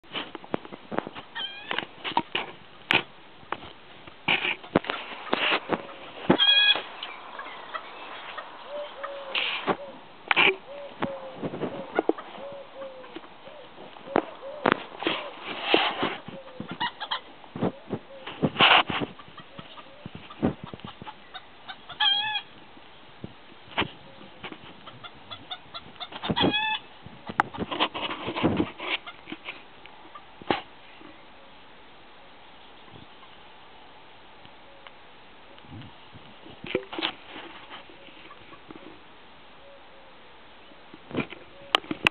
Rocky, Babs and Ginger
Settling in well to their new home... Rocky's just warming up here with the crowing.